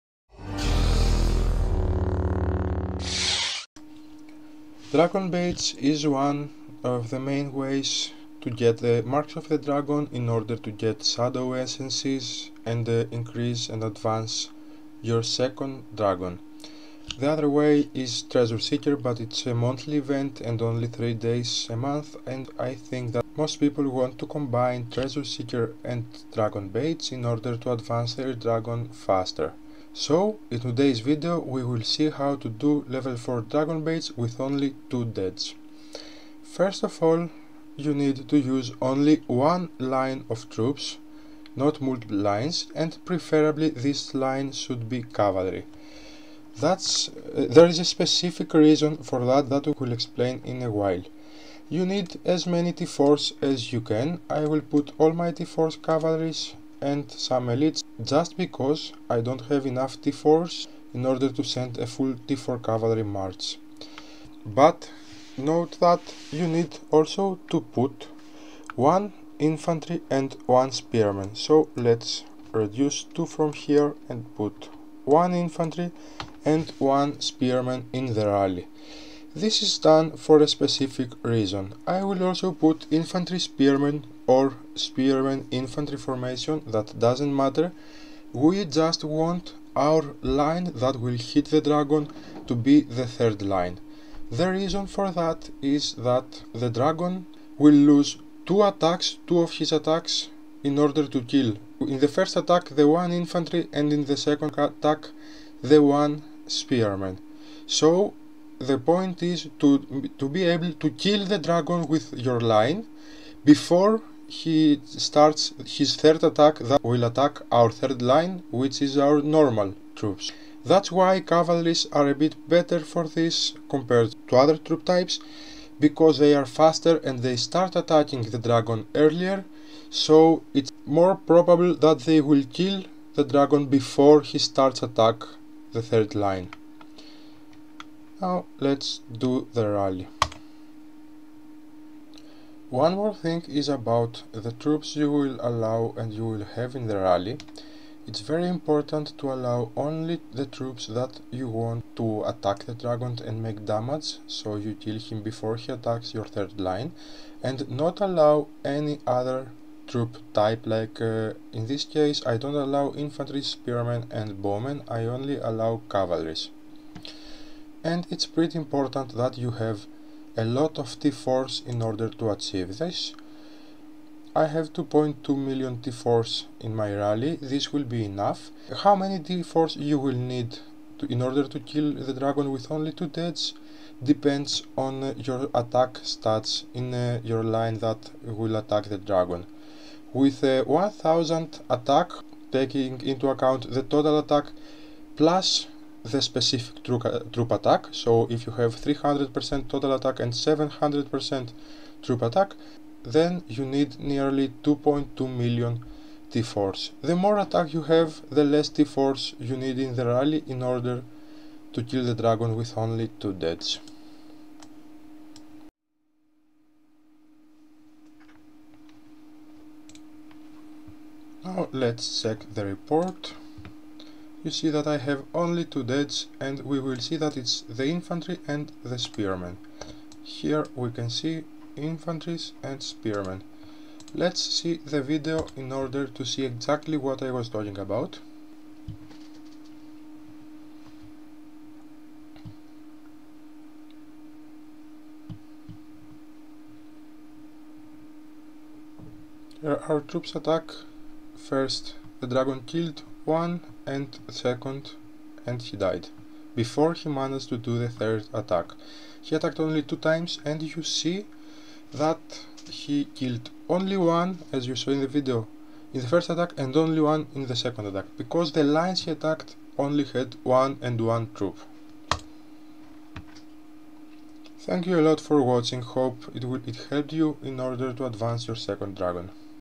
How to kill lvl4 dragon baits with only 2 deads!
Dragon baits is one of the ways to get marks of the dragon, in order to buy shadow essences and advance the second dragon.

The higher the lvl of the dragon bait you kill, the more rewards you get on average.

But this should be with the minimum losses!

In this video I explain how to do lvl4 dragon baits with only 2 deads!!
The trick explained works also on lvl3 for rally leaders with lower stats.